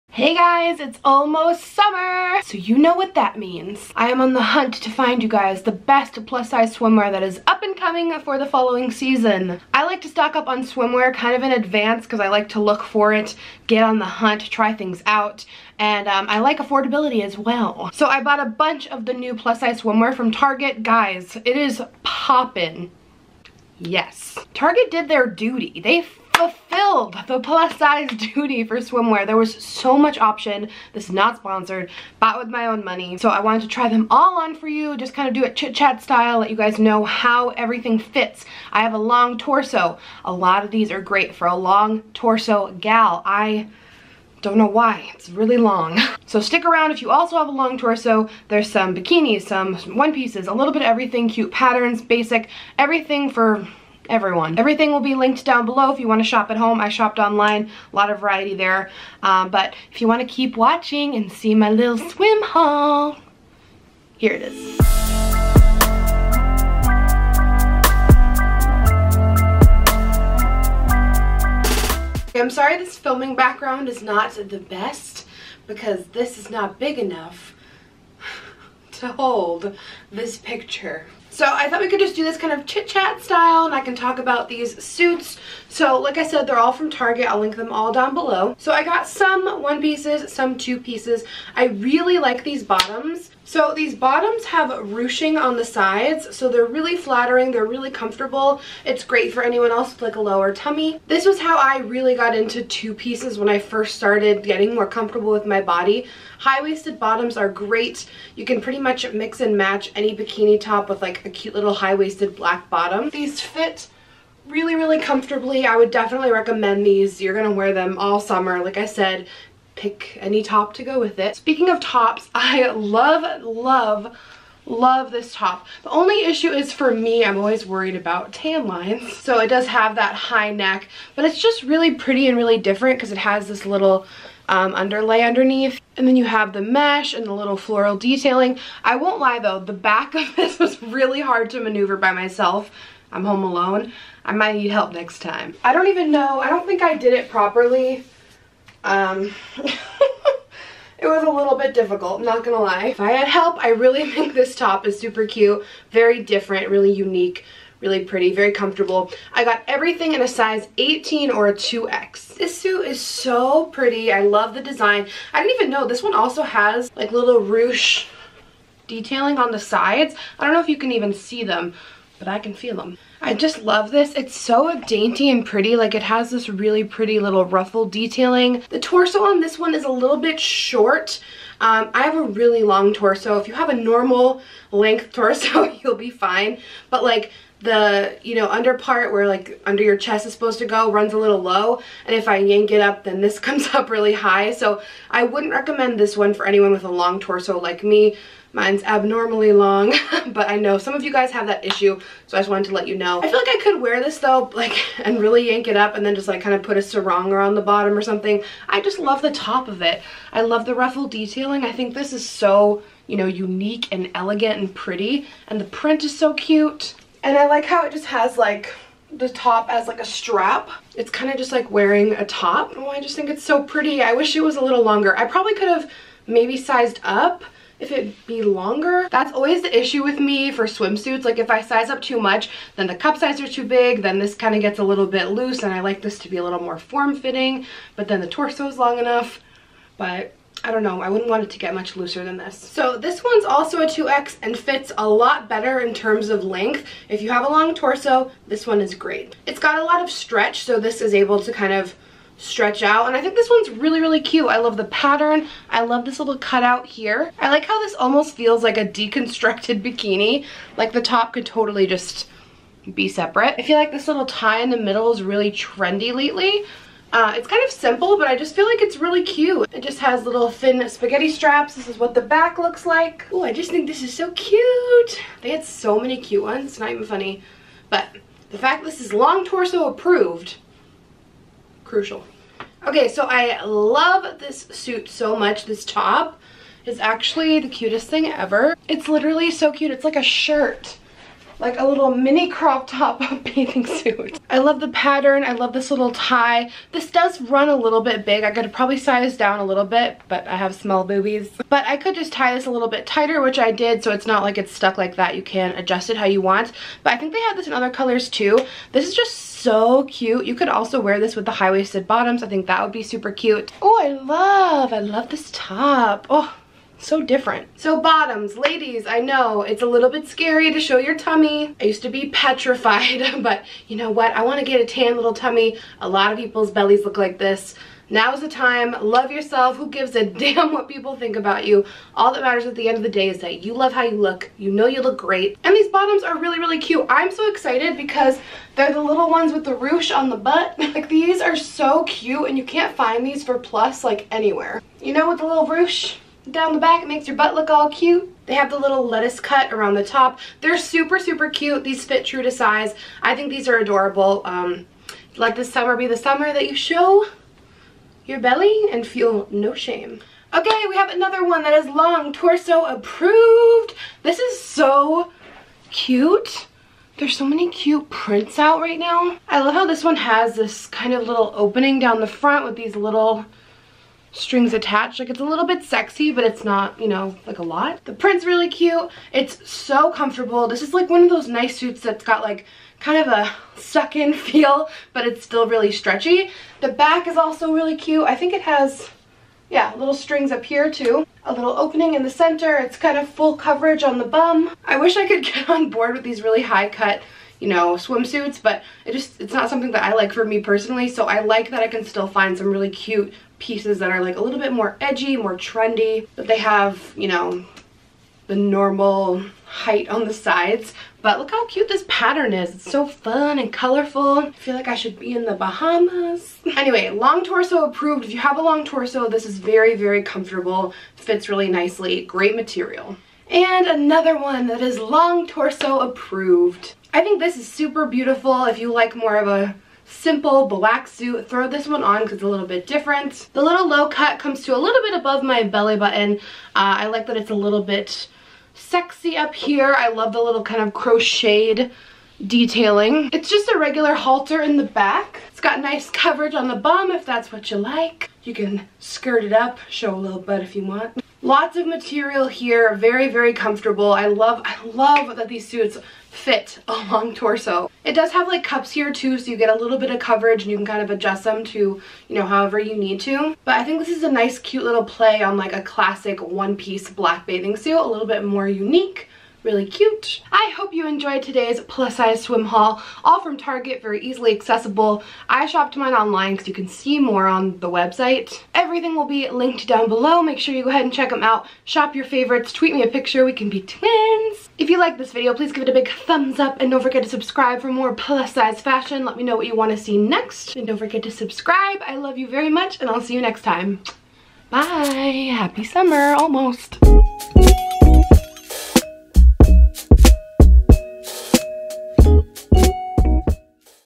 AFFORDABLE PLUS SIZE SWIMWEAR | Haul + Try-On
SUITS SHOWN (IN ORDER):
black mesh floral bikini top
black high waisted bottoms
paisley ruffle one piece
black floral one piece
front tie crop top bikini top
black ruffle bikini bottoms
pink tropical one piece
black one piece

IT'S THAT TIME AGAAAAIN!
I'm on the hunt for the best plus size swimwear for you guys.
I hope you enjoyed this affordable try on haul! Where should we haul next?! xo